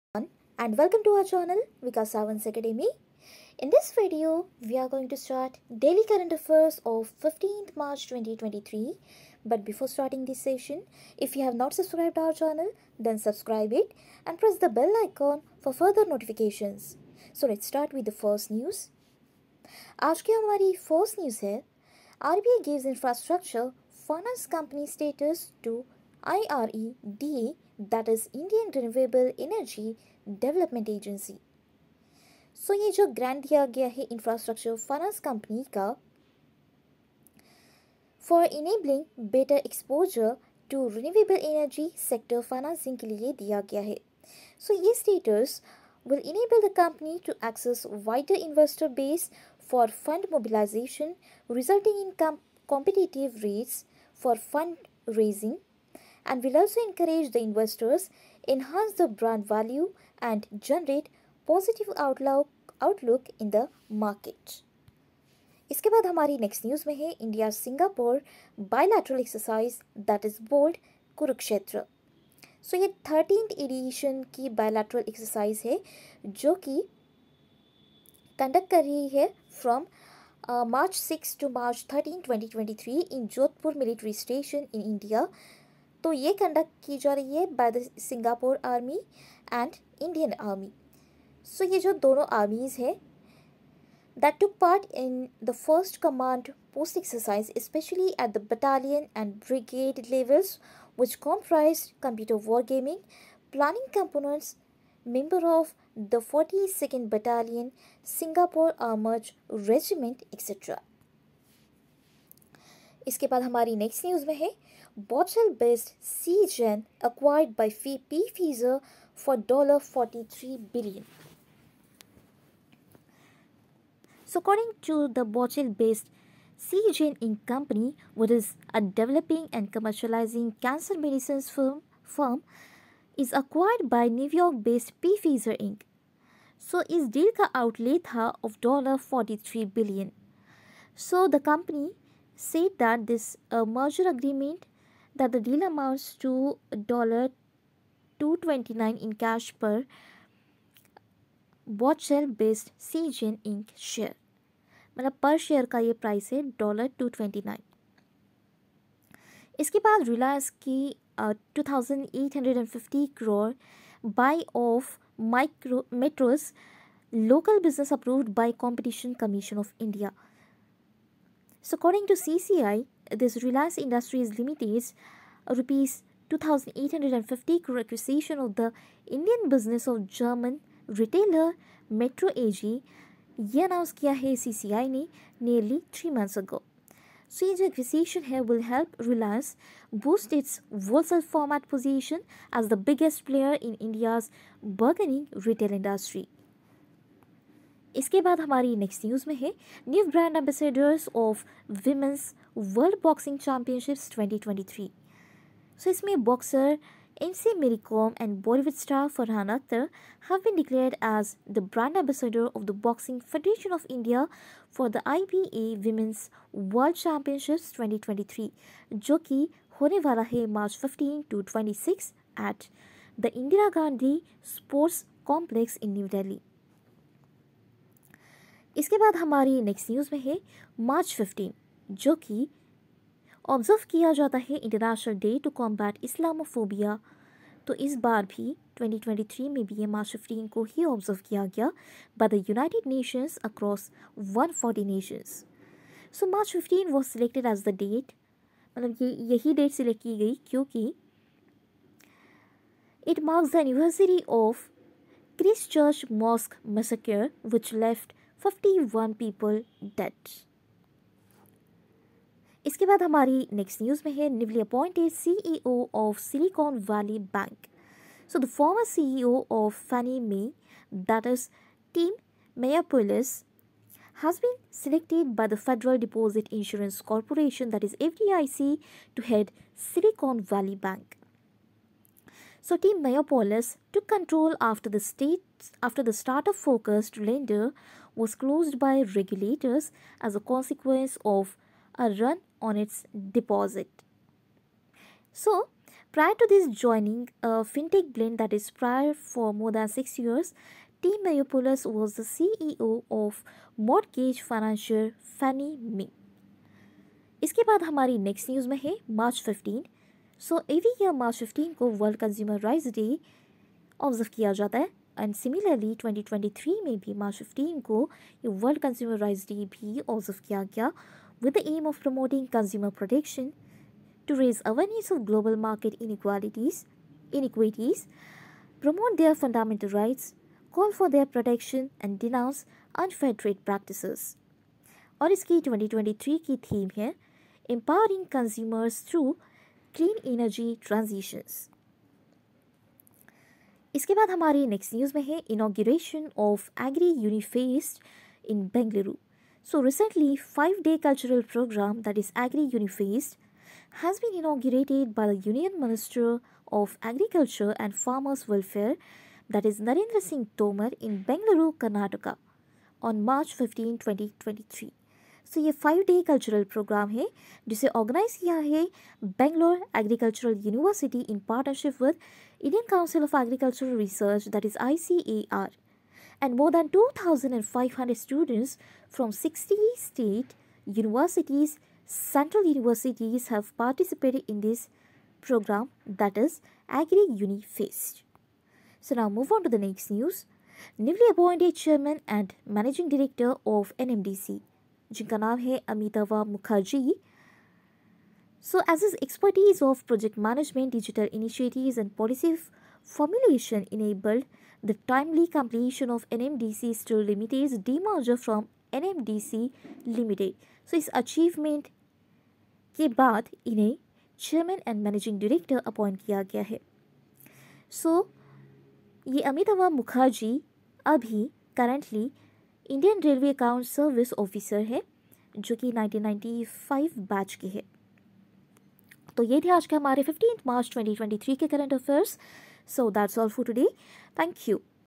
15th March 2023 Current Affairs I Complete Analysis I
15th March 2023 Current Affairs: In Todays Current Affairs session we will discuss daily current affairs MCQ, Important Current Affairs for All Competitive Exams Preparation.

To download Vikas Sawant's Academy App Click on This Link- Only for Android Users: